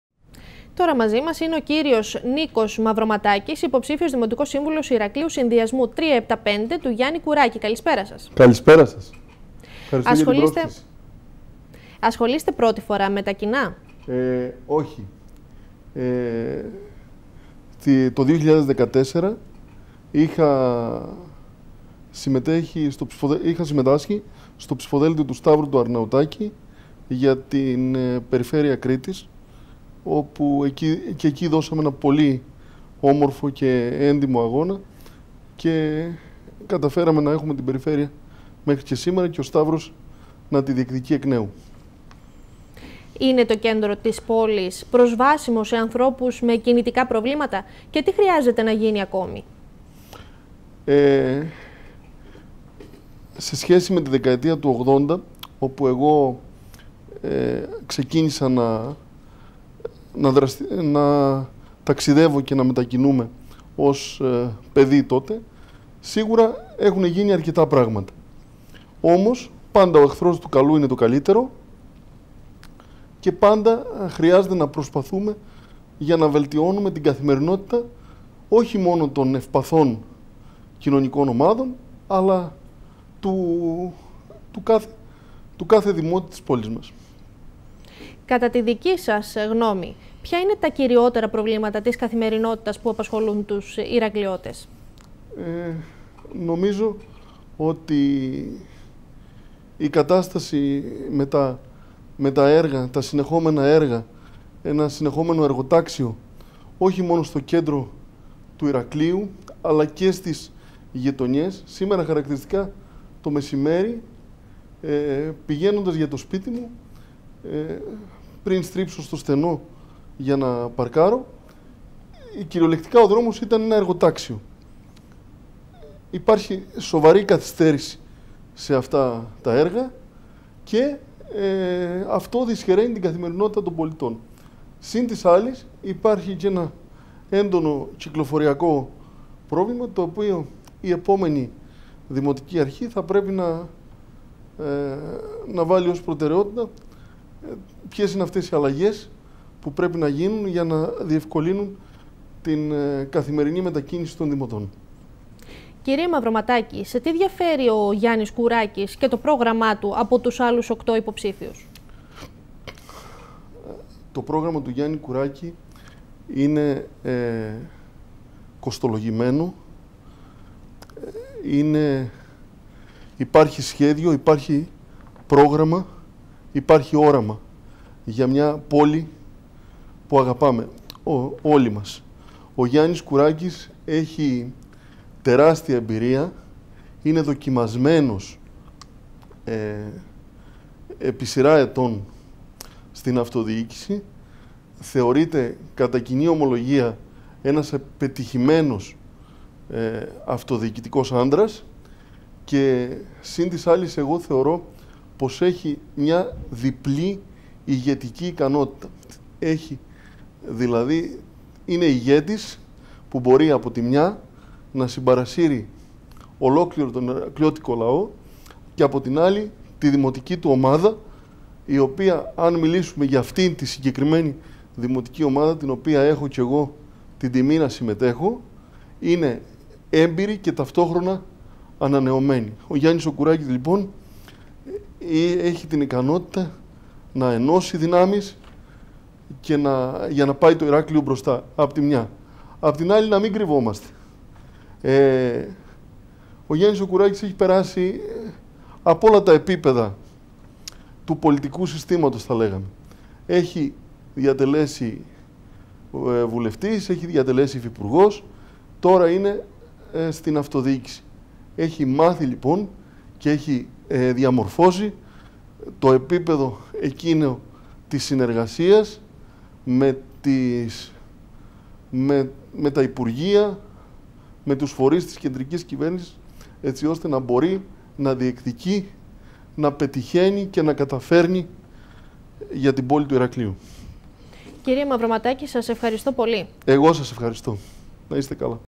NotosNews - Νίκος Μαυροματάκης
Καλεσμένος μας στο δελτίο ειδήσεων του NotosTv ο Νίκος Μαυροματάκης , υποψήφιος δημοτικός σύμβουλος Ηρακλείου με το συνδυασμό ''3.7.5.''